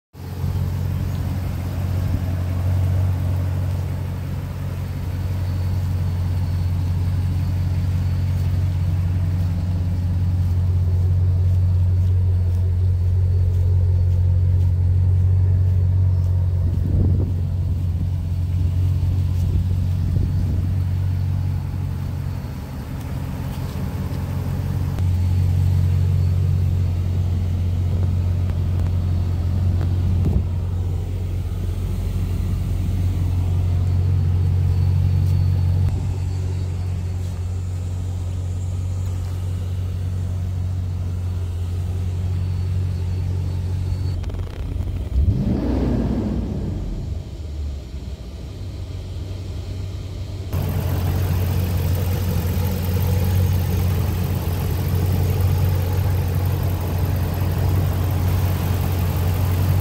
2010 Ram 2500 SLT STK#1344 // Replica Auto Sales
*Reconstructed*
STK # 1344
Key # 89
2010 Ram 2500 SLT
5.7L V8 Auto 4x4
80,730 miles
Reconstructed and ready to drive!
$14,495